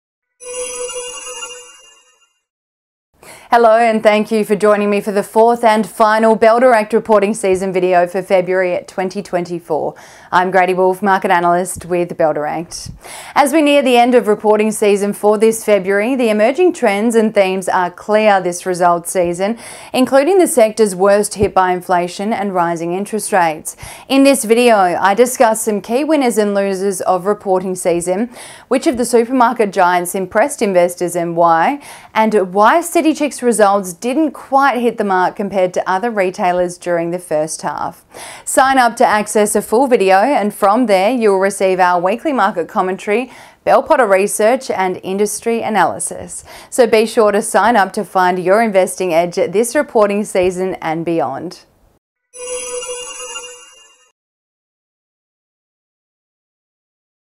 Reporting Season February 2024 Week 4 - Trailer
As reporting season draws to a close, investors await the final results from many companies. Is price gouging really happening between the two supermarket giants? What does Healius' (ASX:HLS) poor results mean in the current market environment? Catch all the latest updates with Bell Direct to help you find your investing edge.

These exclusive insights are only available to investors actively trading with Bell Direct.